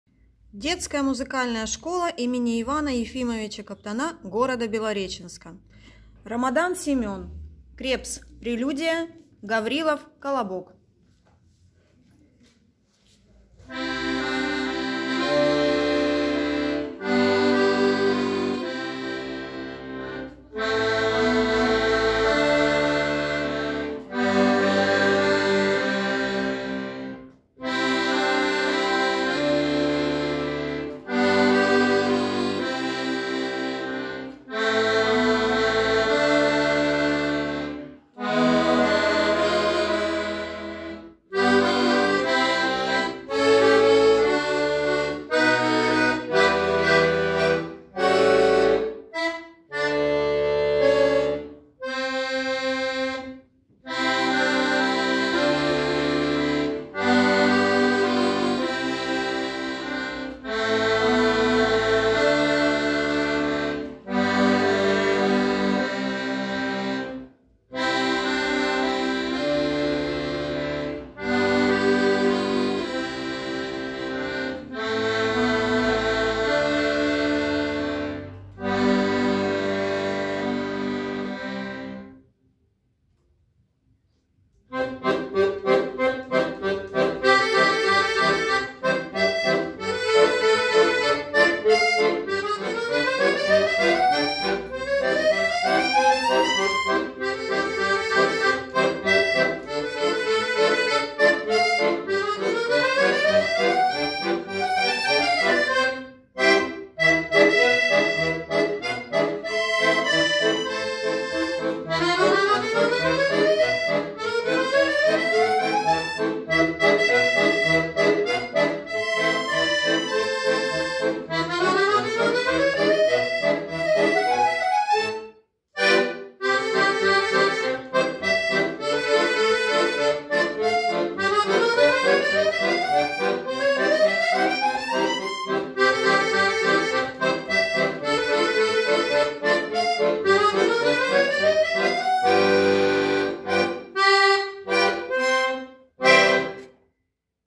Ромодан Семен
Краевой конкурс исполнительского мастерства (народные инструменты) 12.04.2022